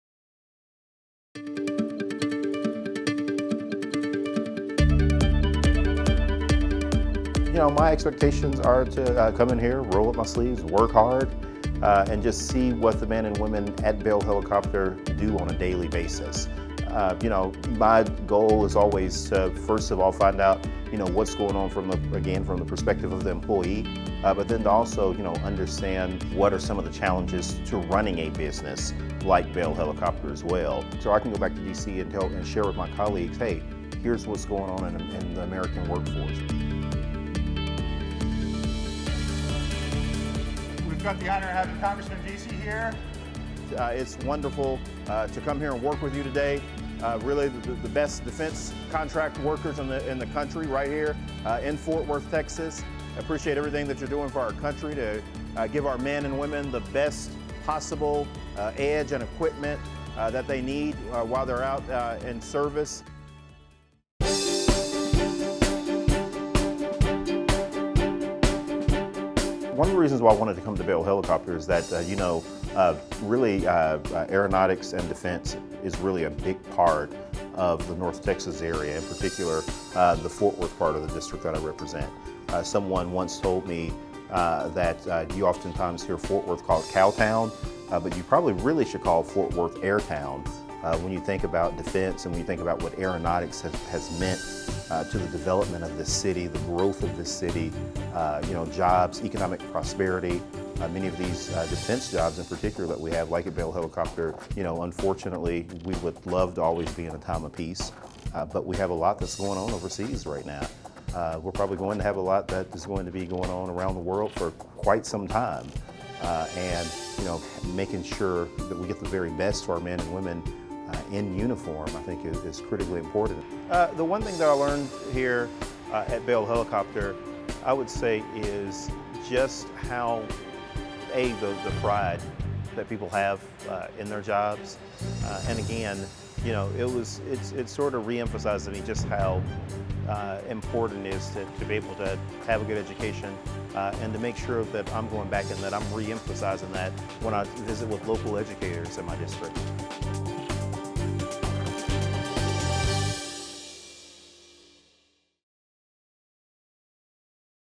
Congressman Marc Veasey Rolls Up His Sleeves at Bell Helicopter
We hosted U.S. Congressman Marc Veasey (TX-33) at our Repair and Overhaul Center as part of his Marc Means Business initiative. Through this ongoing initiative, Rep. Veasey visits and performs work at a variety of businesses across the DFW Metroplex.